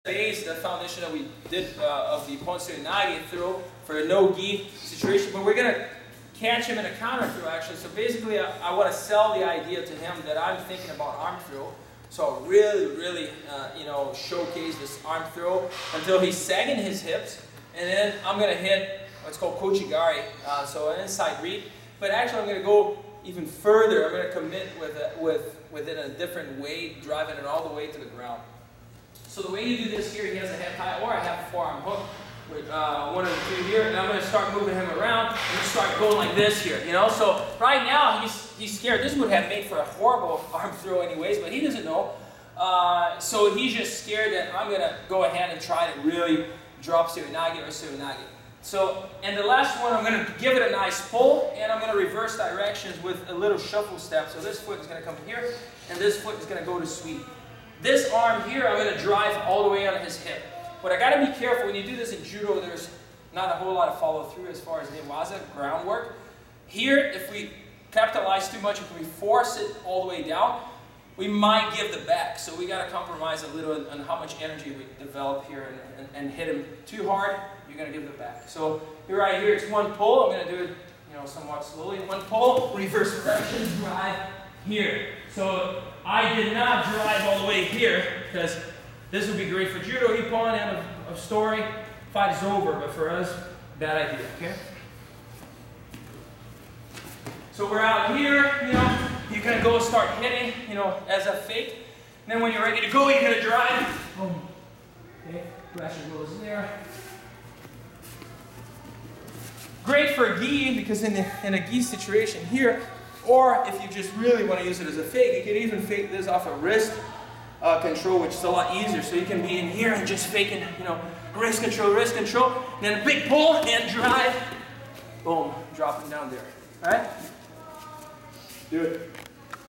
Ippon-Seoi-Nage Fake to Ko-Uchi-Gari/Ko-Uchi-Maki-Komi (No Gi) - [MRJJA IN CLASS 051]
Not good at Judo? No problem!!! Learn how to use an arm throw set-up (Ippon-Seoi-Nage) to fake your opponent into reacting so that you can catch him with a simple minor inside trip (Ko-Uchi-Gari/Ko-Uchi-Maki-Komi).